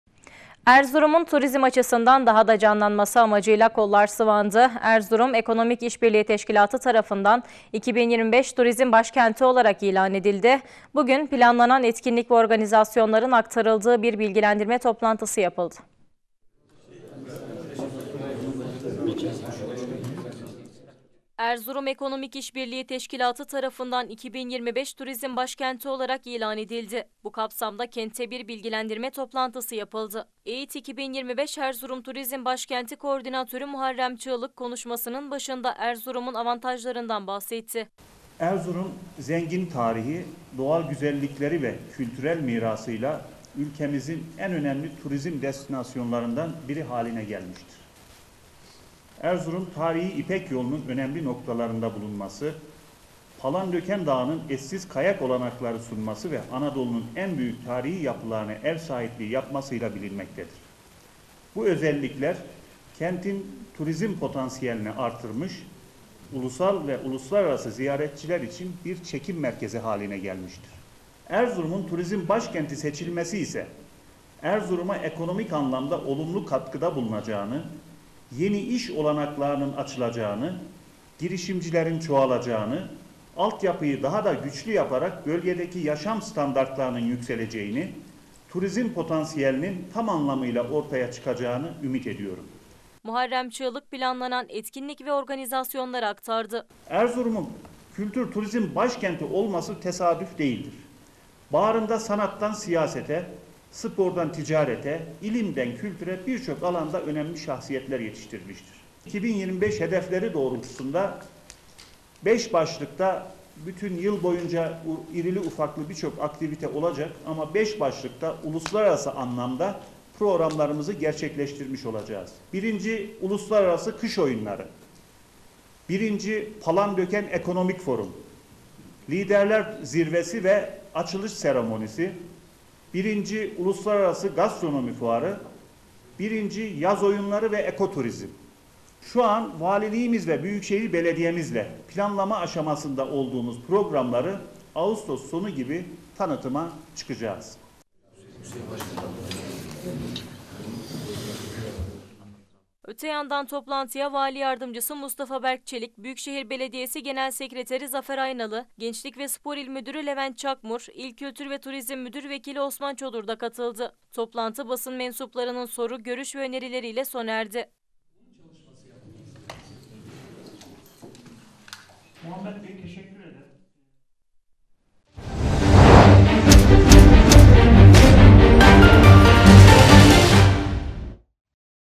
Kardelen TV | ERZURUM, "2025 TURİZM BAŞKENTİ" İLAN EDİLDİ
Erzurum’un Turizm Açısından Daha Da Canlanması Amacıyla Kollar Sıvandı.